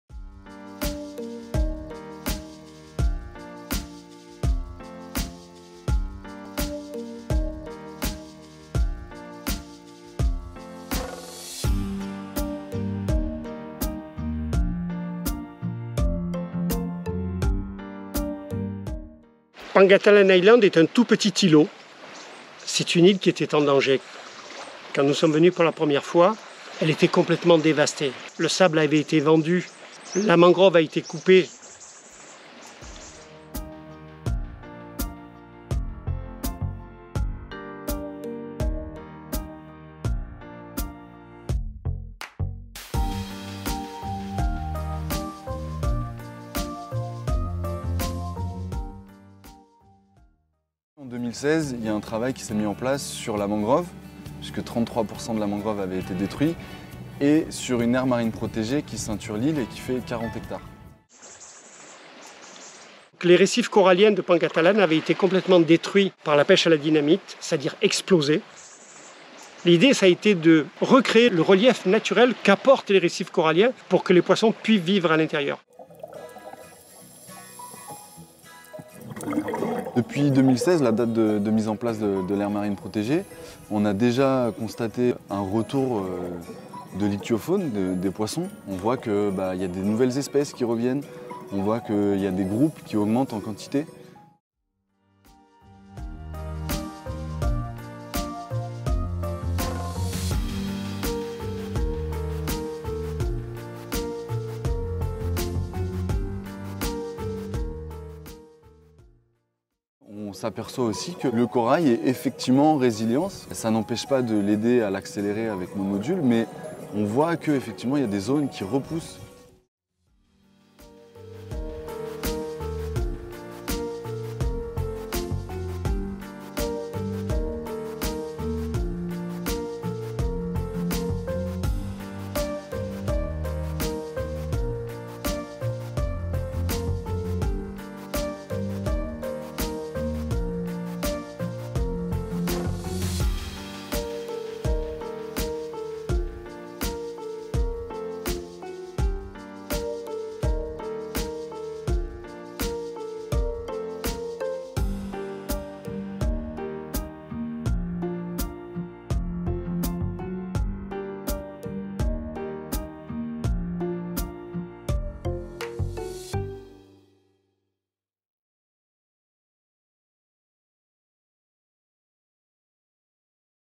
Coral reef restoration in Shark Fin Bay, Philippines
Find out how the FFEM is supporting the Sulubaai Foundation to restore coral reefs and marine biodiversity through the creation of a marine protected area (MPA) and the installation of innovative prostheses for coral reproduction.